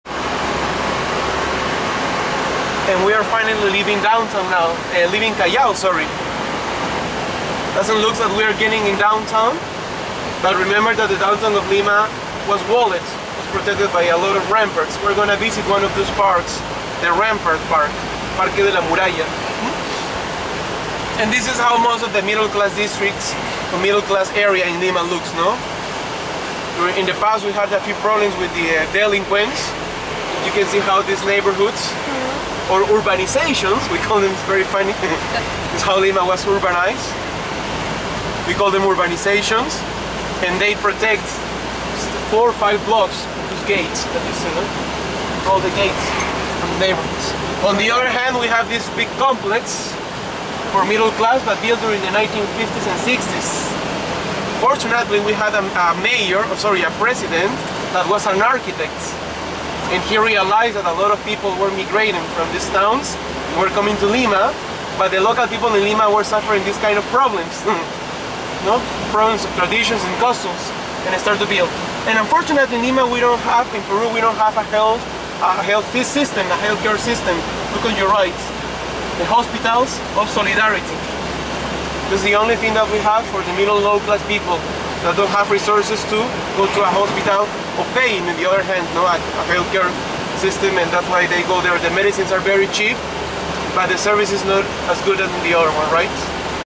South America Dec15 video clip #011 - Lima Peru part 2
Part  2 of a private tour by Hakutours.com of Lima Peru